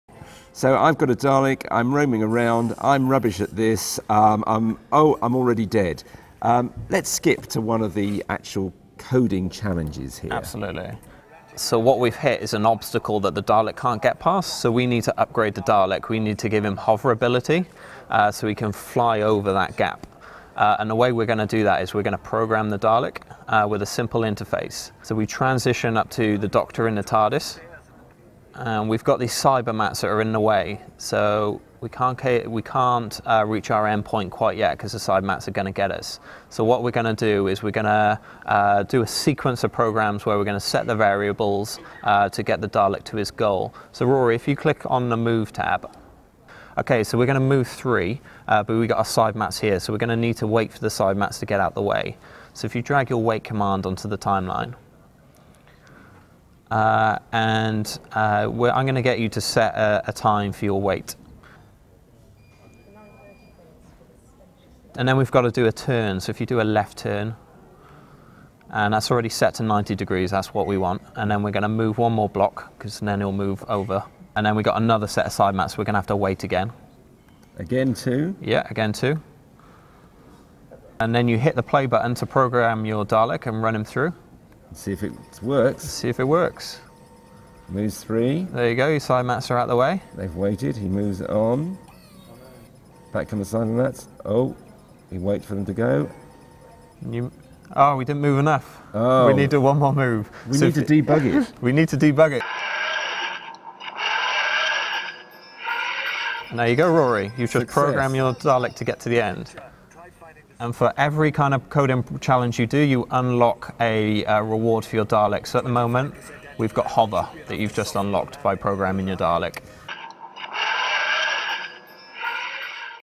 The Doctor and the Dalek - BBC News
‘The Doctor and the Dalek’, a new online Doctor Who game to get more young people into computer coding has been announced.

BBC News technology correspondent Rory Cellan-Jones gives it a try with game producer Richard Jenkins of BBC Wales Online.

Doctor Who 24/7